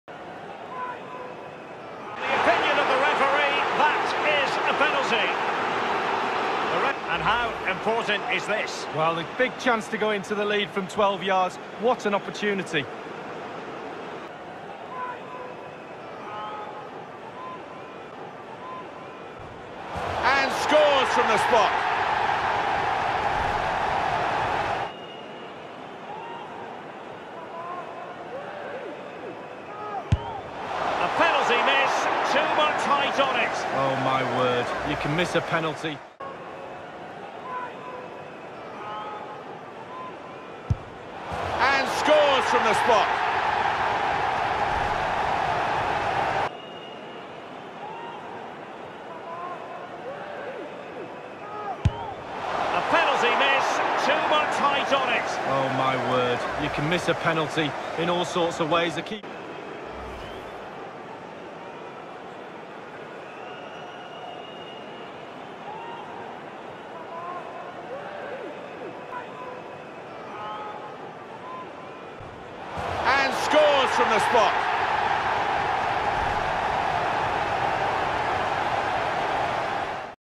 FIFA Penalties In Real Life
In this video I'm going to be recreating fifa 20 penalties in real life.